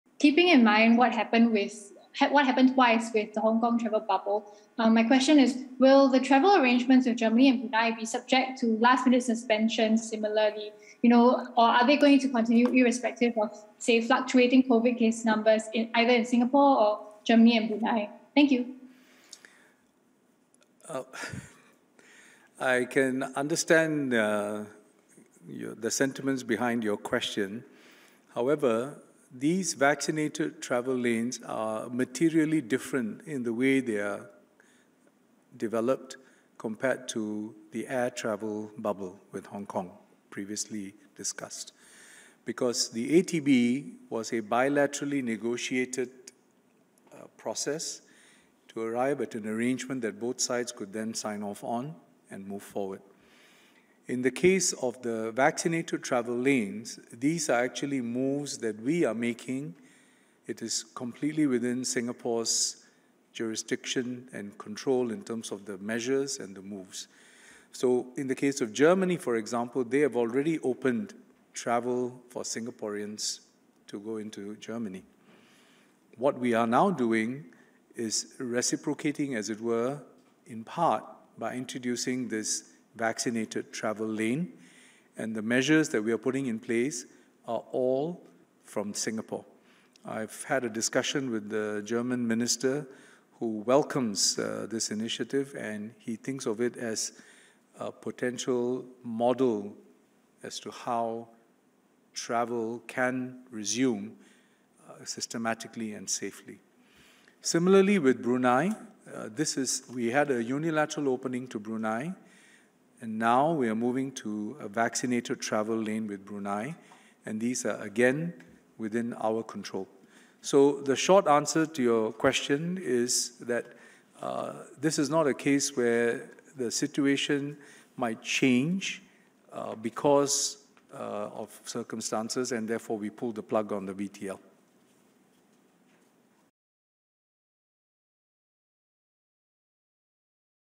Will Singapore’s COVID-19 travel lanes with Germany, Brunei be subject to last-minute suspensions?
Singapore will pilot vaccinated travel lane (VTL) arrangements with Germany and Brunei from September. At a news conference on Thursday (Aug 19), Transport Minister S Iswaran was asked if these would be suspended if COVID-19 situations in these countries changed. “These vaccinated travel lanes are materially different in the way they are developed, compared to the air travel model with Hong Kong previously discussed," he said. "So ... this is not a case where the situation might change because of circumstances and therefore we pull the plug on the VTL."